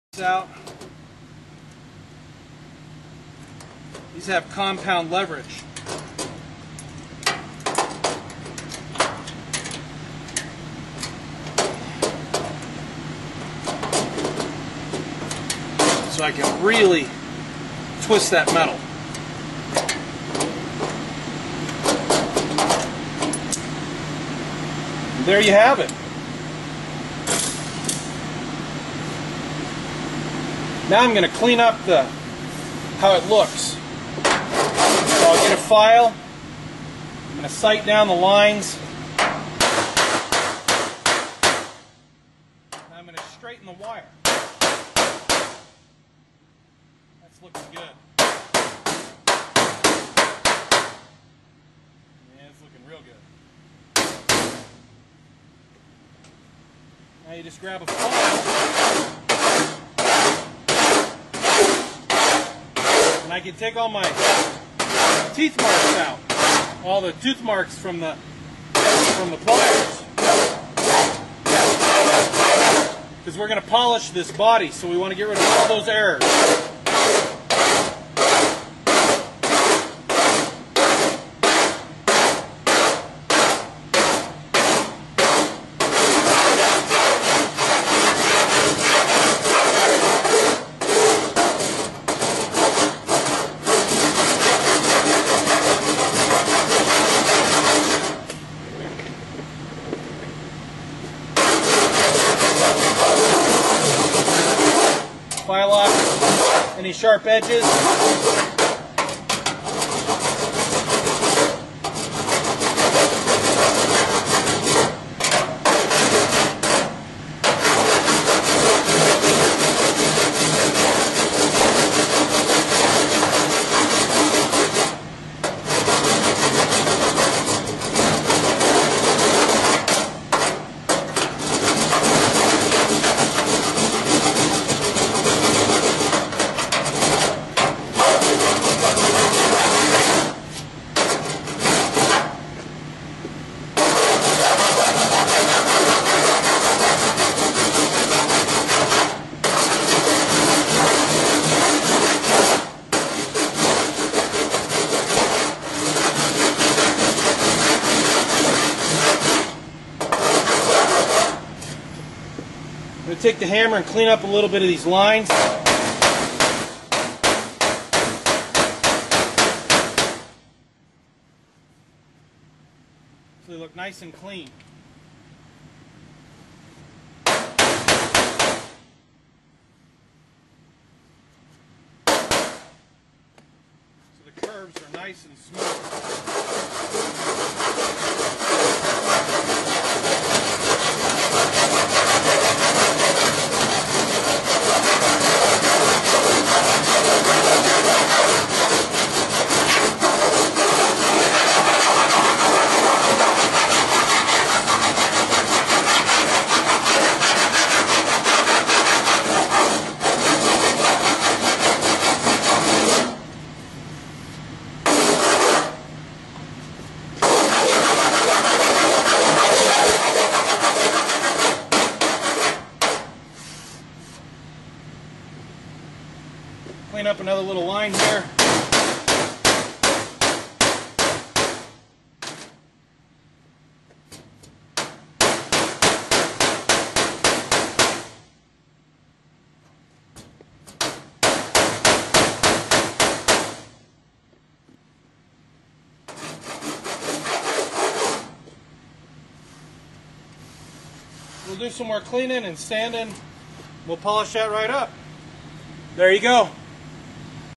Kirkham Motorsports Billet Aluminum Chassis
Kirkham Motorsports wiring front fender 3 of 3 on the Billet Aluminum Chassis